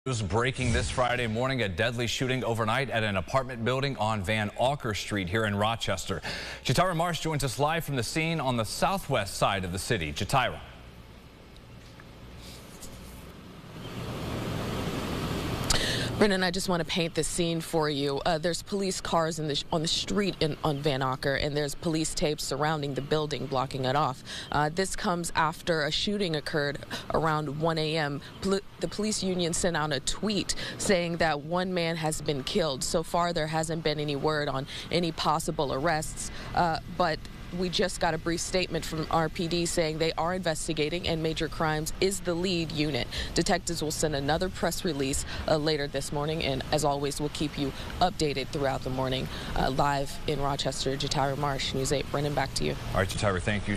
RPD Union: Man shot and killed on Van Auker St.
A man was killed following a shooting that took place at an apartment complex on Van Auker Street early Monday morning, according to the police union.